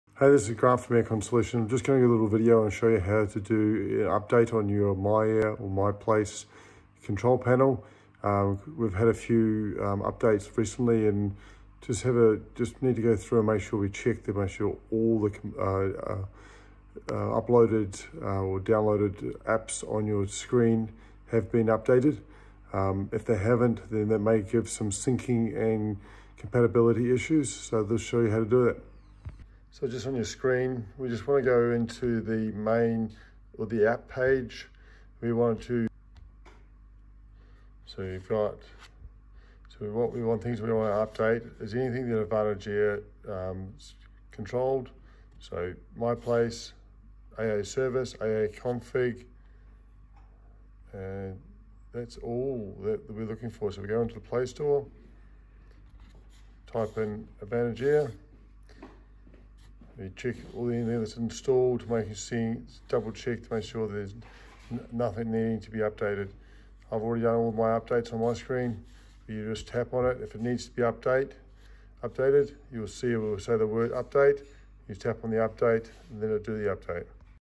MyPlace MyAir Advantage Air
In this video we show how to review and update the system supporting apps for your system.